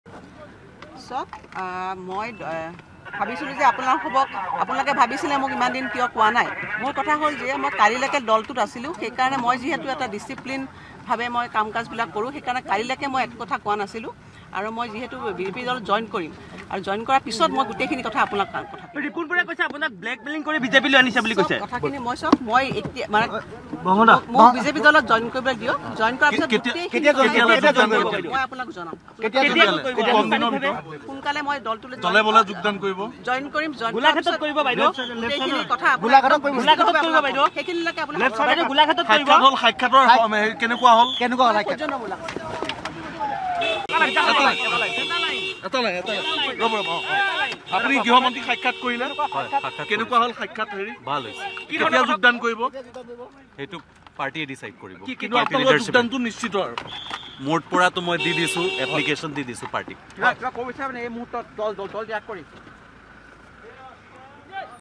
অমিত শ্বাহক সাক্ষাৎ কংগ্ৰেছ বিধায়িকা অজন্তা নেওগৰ || অমিত শ্বাহক সাক্ষাৎ ৰাজদীপ গোৱালাৰ || Exclusive
অমিত শ্বাহক সাক্ষাৎ কংগ্ৰেছ বিধায়িকা অজন্তা নেওগৰ
‘কংগ্ৰেছত থকাৰ বাবে মই একো কোৱা নাছিলোঁ’
বিজেপিত যোগদান কৰি মই সকলো ক’ম : অজন্তা নেওগ
অমিত শ্বাহৰে সাক্ষাৎ সৌজন্যমূলক আখ্যা অজন্তাৰ
অমিত শ্বাহক সাক্ষাৎ ৰাজদীপ গোৱালাৰ
‘অচিৰেই মই বিজেপিত যোগদান কৰিম’
অমিত শ্বাহক সাক্ষাৎ কৰি ঘোষণা গোৱালাৰ
কেতিয়া যোগদান কৰিম সেই সিদ্ধান্ত ল’ব দলে
অমিত শ্বাহক সাক্ষাৎ কৰি সুখী ৰাজদীপ গোৱালা